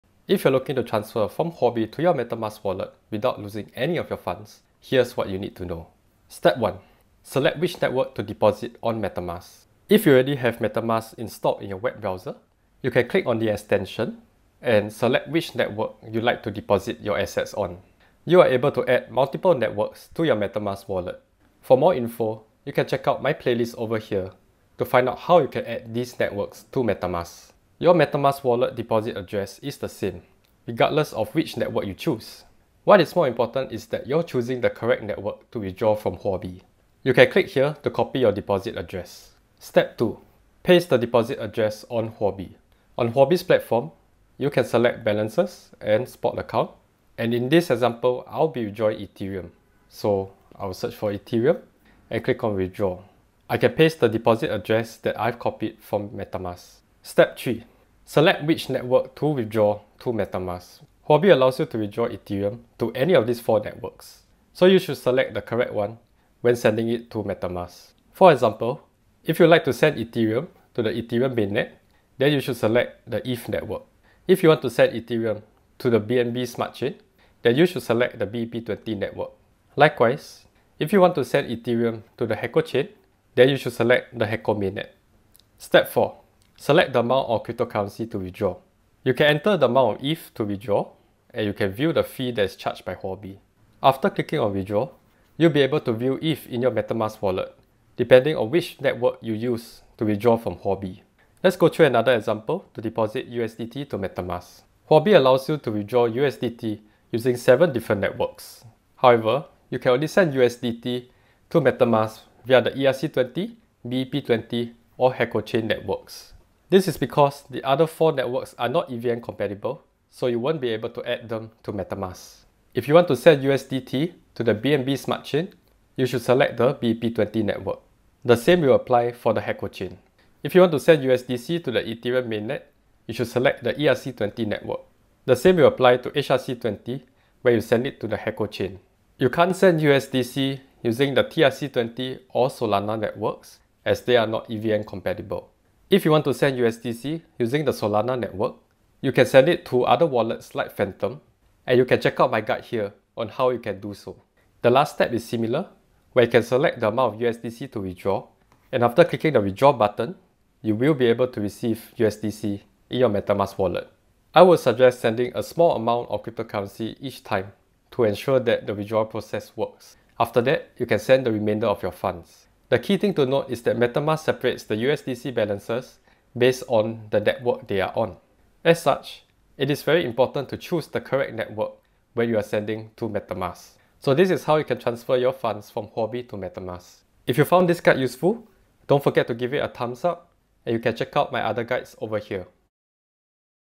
Huobi To Metamask (IN 4 STEPS)
Here's how you can deposit assets from Huobi to your Metamask wallet.

The most important thing is to make sure that you're using a common network between both platforms. Otherwise, your funds may be lost!

Some examples of cryptocurrencies that I went through:
• ETH
• USDT
• USDC

- TIMESTAMPS -
0:00 Intro
0:06 1. Select a common network between Huobi and Metamask
0:32 2. Choose the correct network on Huobi
0:56 3. Select the network to withdraw to Metamask
1:25 4. Select the amount of cryptocurrency to withdraw
1:41 Example 2 - Withdrawing USDT to Metamask
2:10 Example 3 - Withdrawing USDC to Metamask
2:44 Conclusion